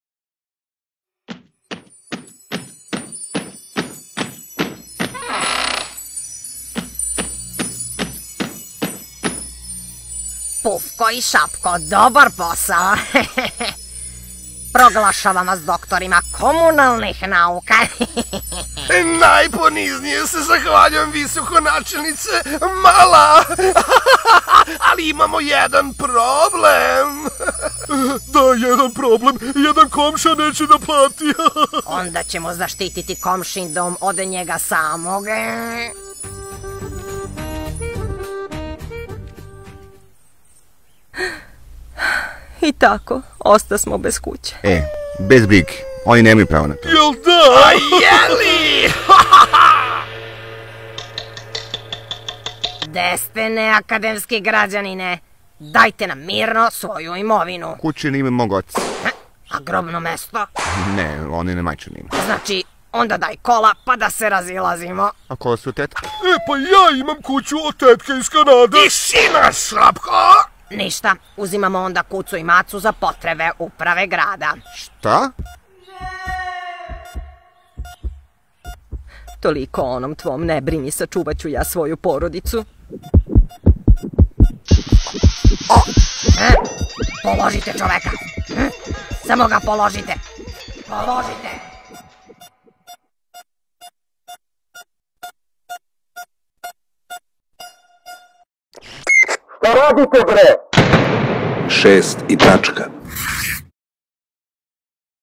Komunalni fantomi u epizodi lubenica
Kratko uputstvo kako zaštititi imovinu od izvršitelja i komunalnih fantoma